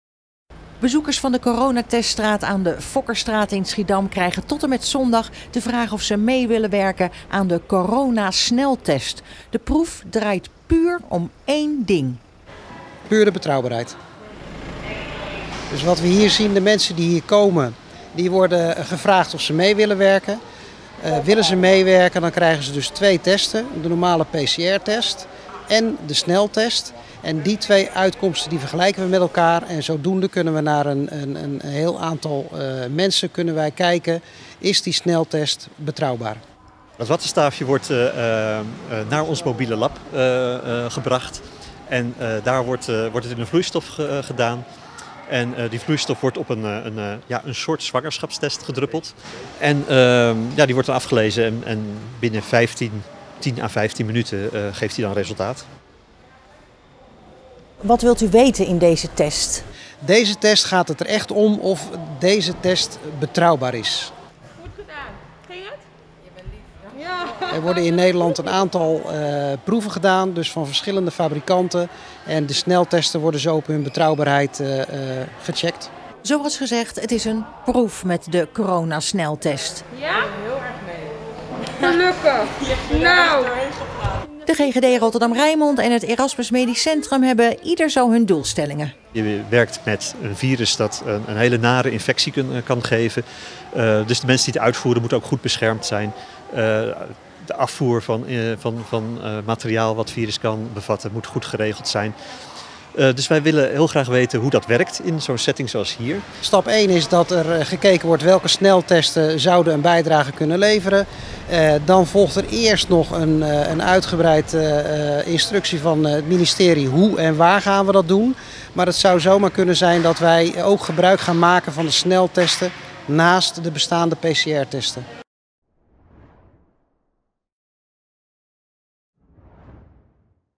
Sneltesten nu ook (even) mogelijk in Schiedam: 'Binnen vijftien minuten resultaat'
In Schiedam wordt, in navolging van Breda en Utrecht, sinds vrijdag proefgedraaid met coronasneltesten. Dat gebeurt in de Fokkerstraat bij mensen die daar naast de reguliere coronatest toestemming voor geven. De GGD Rotterdam-Rijnmond en het Erasmus MC hopen antwoord te krijgen op een aantal vragen.Dan gaat het bijvoorbeeld om hoe deze sneltest zich verhoudt tot de normale test. &quot;Qua gevoeligheid en vals positieven. We weten al een beetje dat de sneltest qua gevoeligheid iets minder is, maar de vraag is of dat verlies opweegt tegen de snelheid&quot;, zegt Richard Molenkamp, viroloog van het Erasmus MC. Een andere belangrijke vraag is of de gekozen setting daadwerkelijk toepasbaar is. In Schiedam is een mobiel lab opgesteld in de al bestaande testlocatie. Molenkamp: &quot;Hier vergelijken wij de sneltest met de PCR-test. De normale tests gaan via een koerier naar het laboratorium in het Erasmus MC. Dat kost tijd met transport en de test zelf duurt langer. Bij een sneltest wordt het monster ter plaatse onderzocht. Het zou mooi zijn als een sneltest een bijdrage levert aan de versnelling om te kijken of iemand positief of negatief is.&quot;Voor de sneltest wordt in Schiedam bij mensen een extra swap afgenomen via de neus. &quot;De animo is groot&quot;, zegt Roel Ensing van de GGD Rotterdam-Rijnmond tevreden. &quot;Binnen 15 a 30 minuten is het resultaat te zien. Hoe de test verder ingezet gaat worden als er groen licht komt, is aan het ministerie. Het is te vroeg om daarop vooruit te lopen.&quot; Betrouwbaar&quot;Wij willen vandaag antwoord krijgen op de vraag hoe betrouwbaar de test is, daarna wordt nagedacht en gebrainstormd over hoe het testlandschap eruit gaat zien&quot;, vervolgt Ensing. &quot;Als je meer met sneltesten gaat werken zit daar logistiek wel een apparaat achter dat moet werken. Maar dat is niet aan ons.&quot;Maar hoe werken de sneltesten? &quot;Er wordt een naast de normale test een tweede swap uit de neus genomen en die gaat meteen naar het mobiele laboratorium. Daar wordt die swap in een vloeistof geplaatst, die speciaal voor de sneltest bedoeld is. Van de vloeistof worden een paar druppels in een sneltestcasette gedaan. Allemaal met goede bescherming en een goede afvoer van het afval dat het virus kan bevatten&quot;, legt Molenkamp uit. &quot;De samples die positief uit de test komen zetten wij op virale kweek. Wij bepalen ook of het virus infectieus is.&quot;Molenkamp vervolgt: &quot;We zijn nu aan het proberen, maar het gaat erg voorspoedig. Maar als je echt zou willen opschalen, maak je misschien andere keuzes. Het is een sneltest, maar je hebt nog steeds veel personeel om het uit te voeren. Het vergt veel handwerk.&quot;